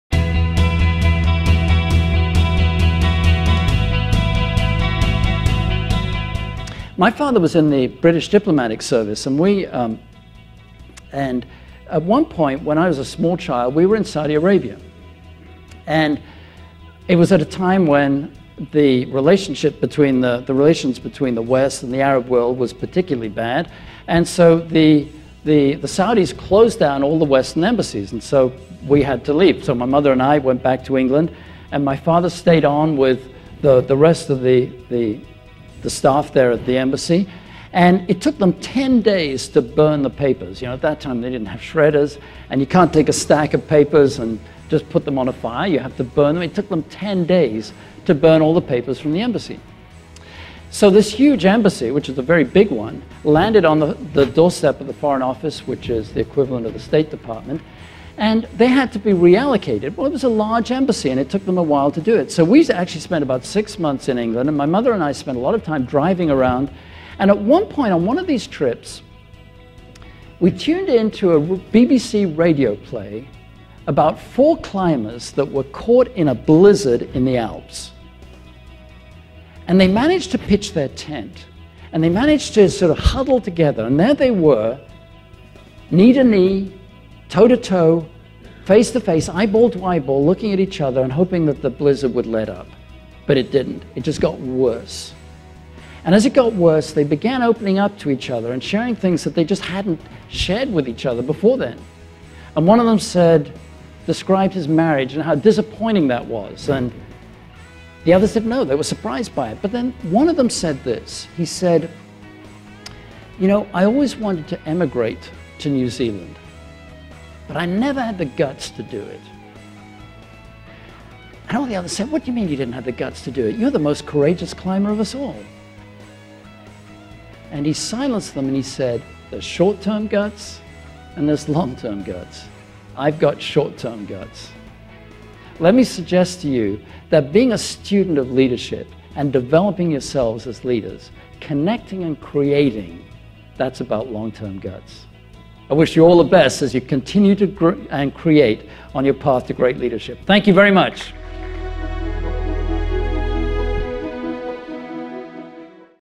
Great Leaders: Short-Term Guts or Long-Term Guts?
It takes time to develop into a great leader.  But patience and persistence pays off.